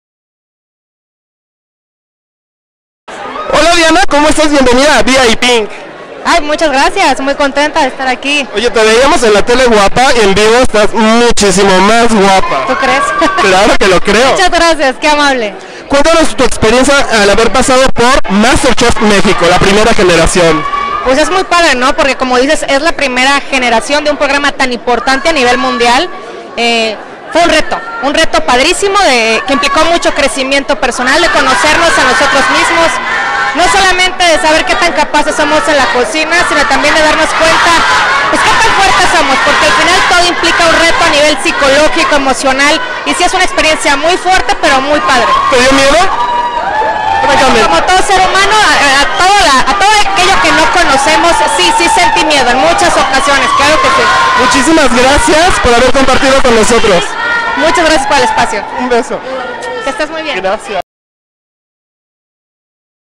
Vipink/Diana Abreu de Master Chef México
Diana Abreu nos cuenta cono fue su paso por